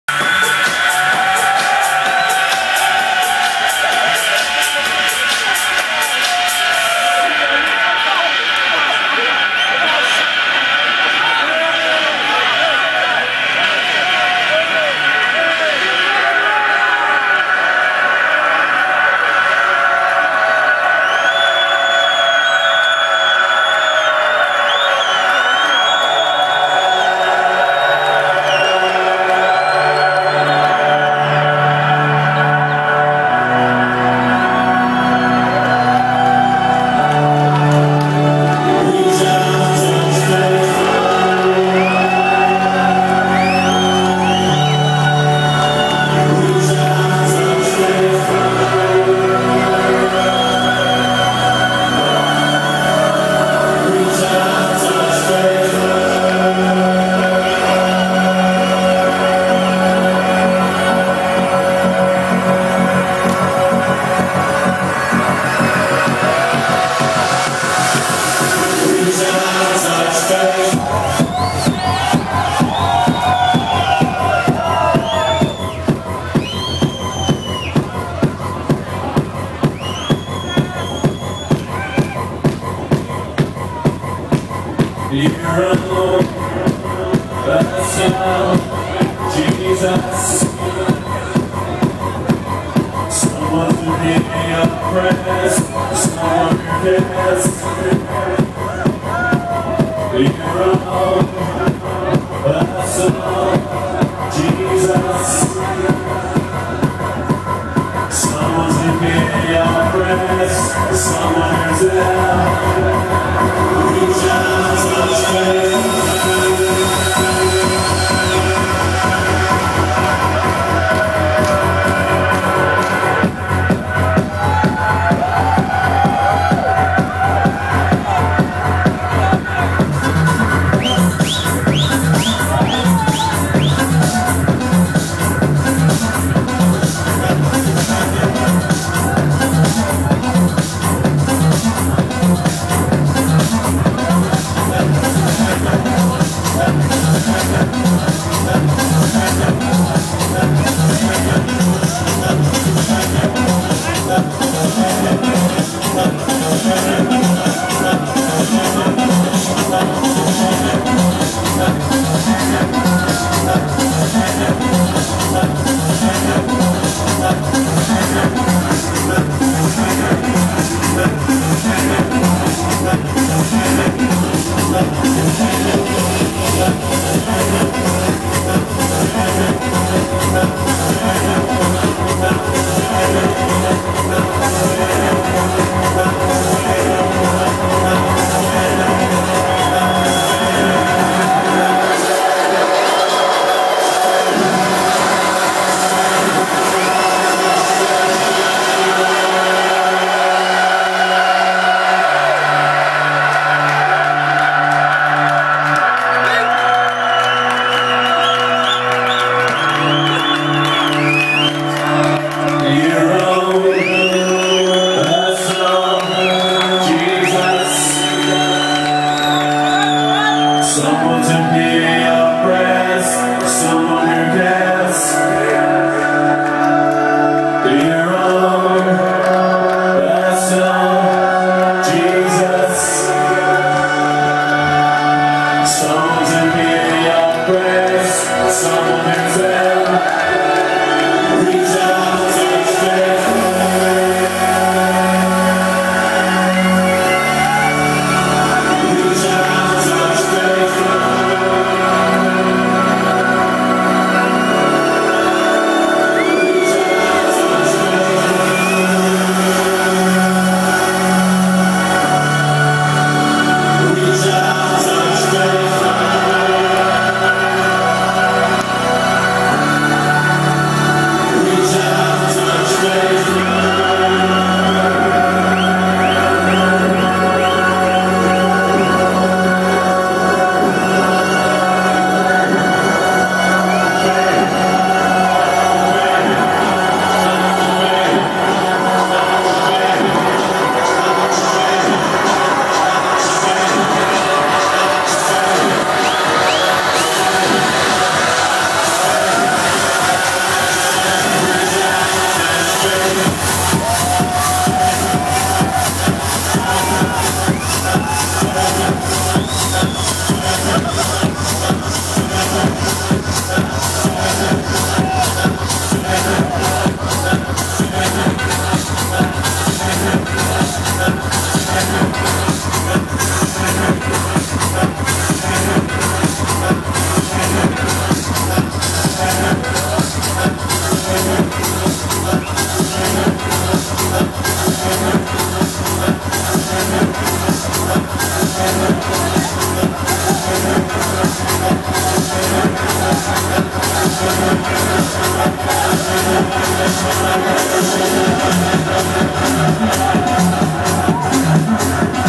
Stefano Noferini at Electronica Festival - Istanbul - Turkey
Stefano Noferini open is set at Electronica Festival in Istanbul - Turkey at Toolroom stage Enjoy!